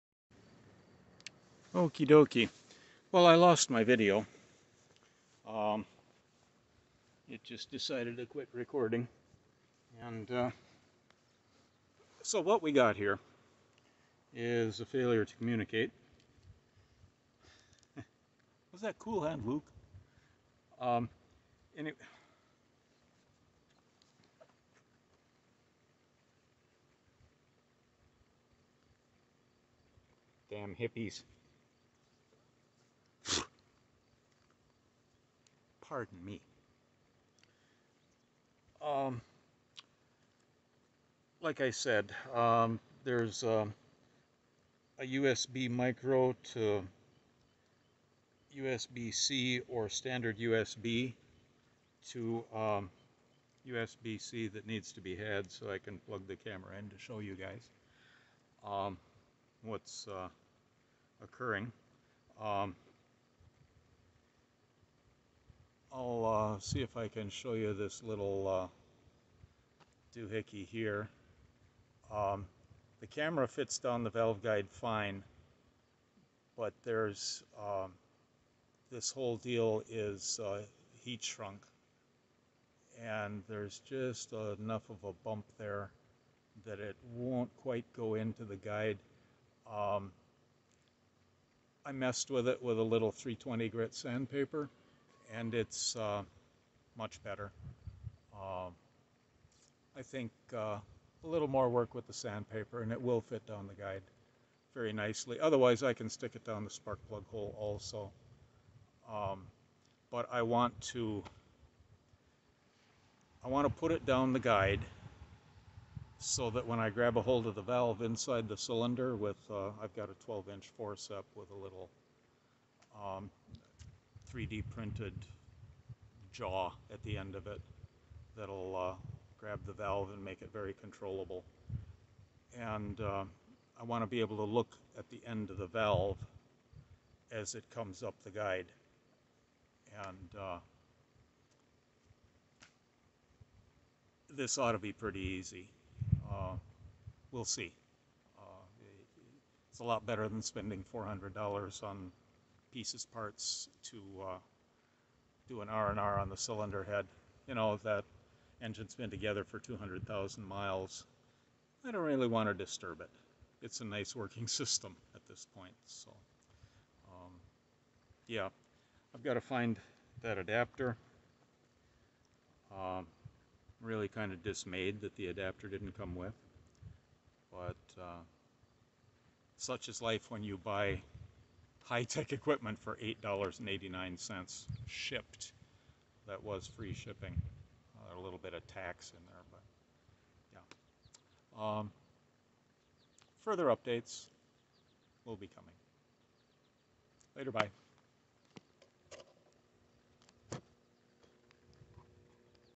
Cheap Borescope Update
I had ordered two cheap borescopes, one for $8-something that plugs into a cellphone.  The other cost $37-ish and has WiFi with a range of roughly 30 feet. (10 meters)

The REAL cheap one showed up first, and was missing an adapter that was promised in the listing.  That's the one you see causing me frustration here.

The WiFi-equipped one arrived a couple days later and, practically set itself up to work with my cellphone...THAT's the one I ended up using.  I've no link, but it was purchased through Walmart's site.  Search 5.5mm borescope/endoscope, and WiFi, a zillion results will show up...just close your eyes and click, that's how I chose mine!